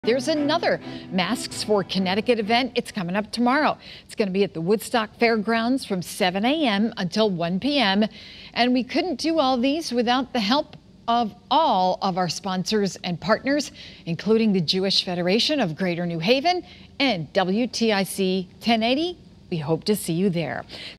VIDEO: Masks for CT giveaway set for Woodstock
Channel 3's 9th Masks for CT giveaway has been scheduled for May 26 in Woodstock.